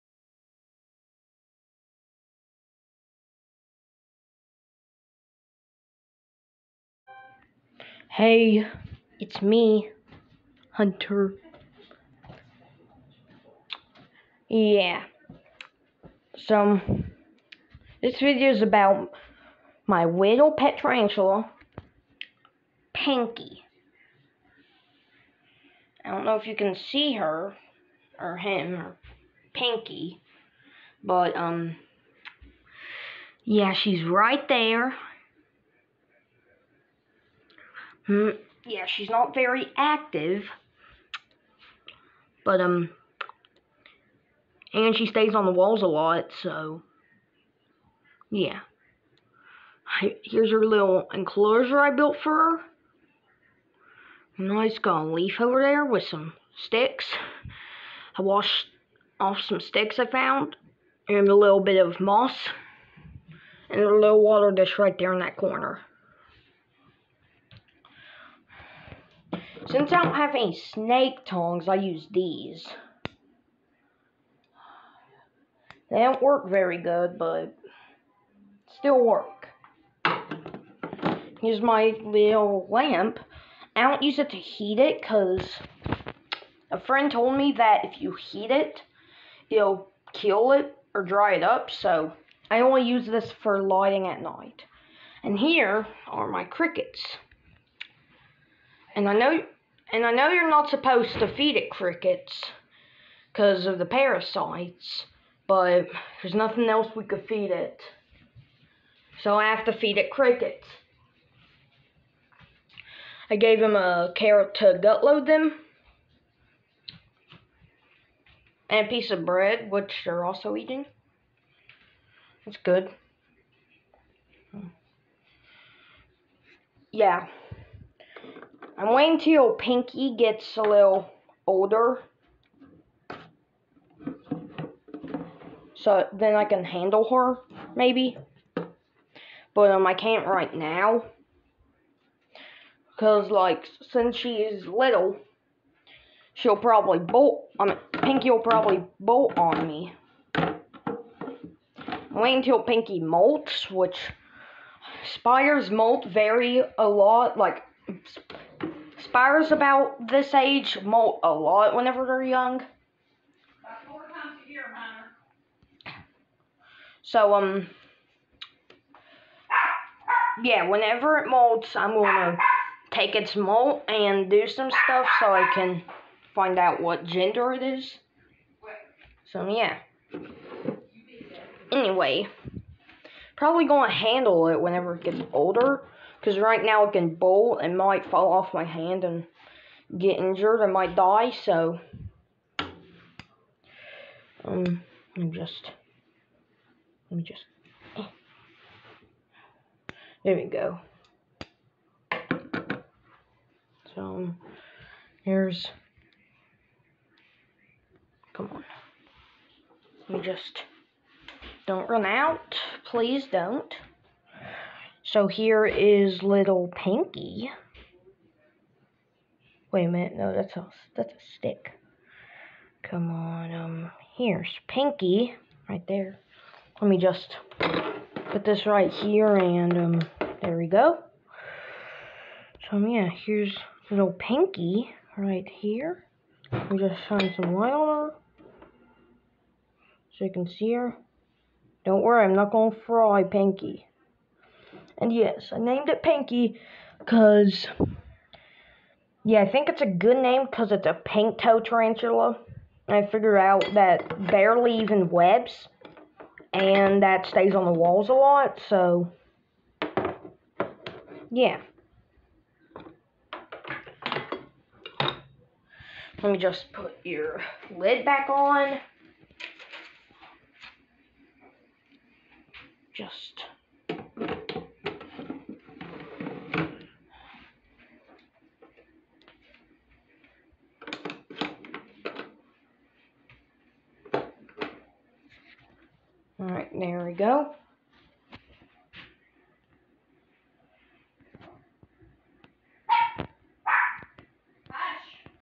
introduction to pinkie (pink toed tarantula
this is my pet tarantula named pinkie (which is a pink toed tarantula).